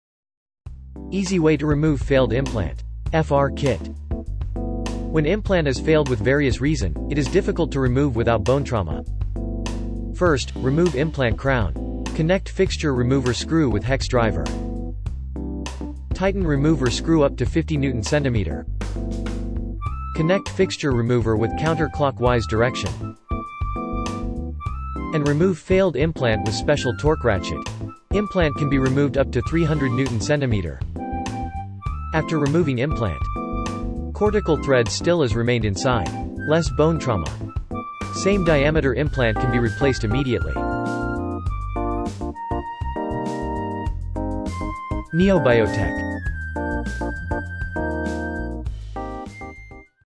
How to remove ailing or failed implant without bone trauma - Fixture Remover Kit (FR Kit)
Remove ailing or failed implant without bone trauma.
After removing the implant, it can be replaced with same diameter implant.
Origin since 2009
Sold out 14000kits in the world.
Many references